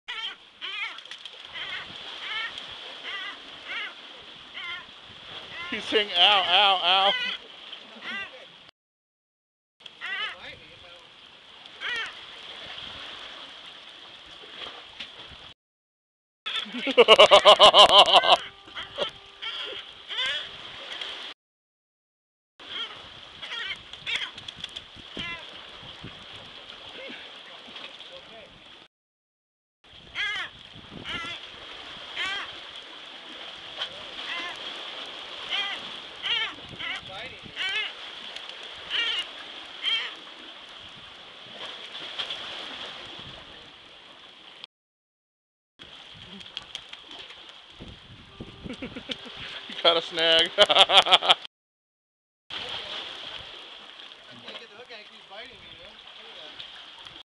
Wild bird gets snagged, and says OOOWWW.
This is a video that I shot when we were fishing on the Oregon Coast and my friend caught a bird that was stealing out bait.  When my friend tried to get the hook out, the bird told us how much it hurt.